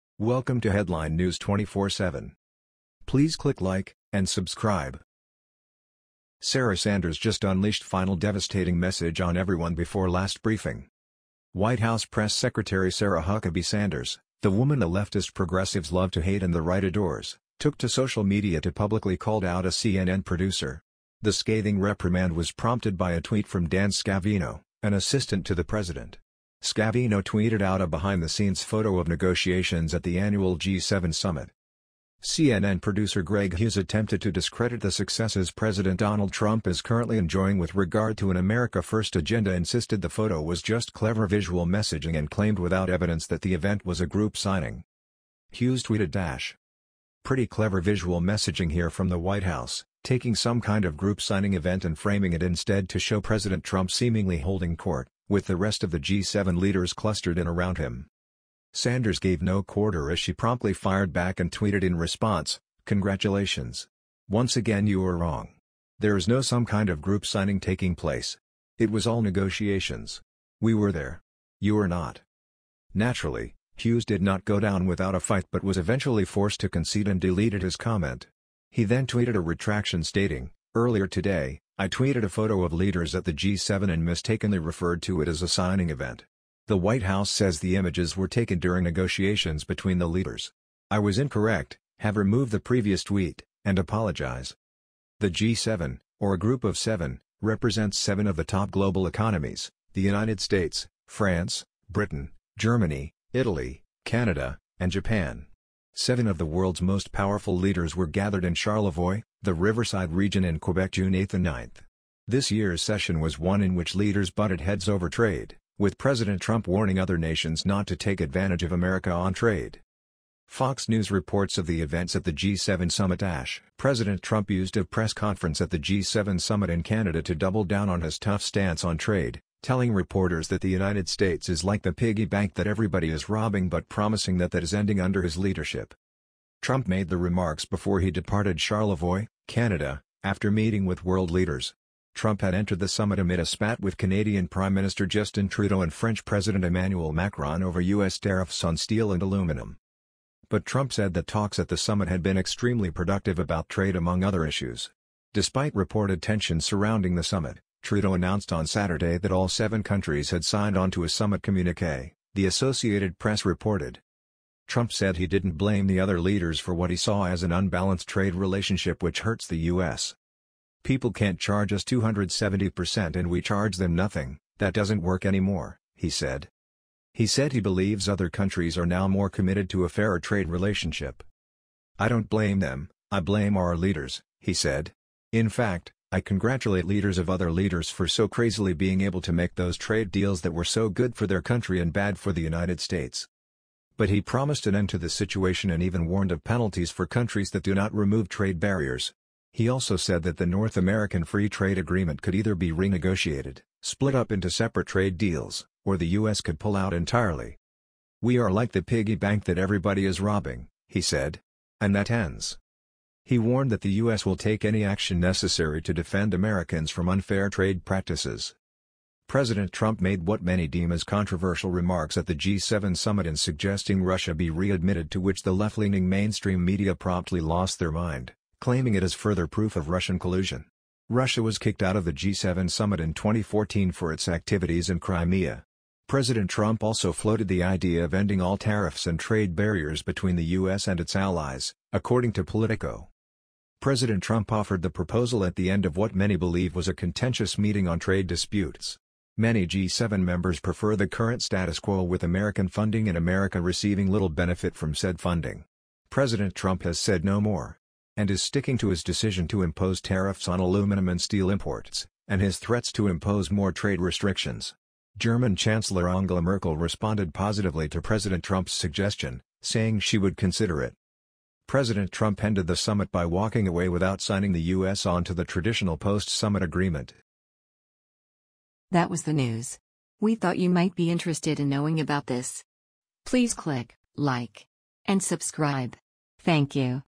Sarah Sanders Just Unleashed Final Devastating Message On Everyone Before Last Briefing
This year’s session was one in which leaders butted heads over trade, with President Trump warning other nations not to take advantage of America on trade.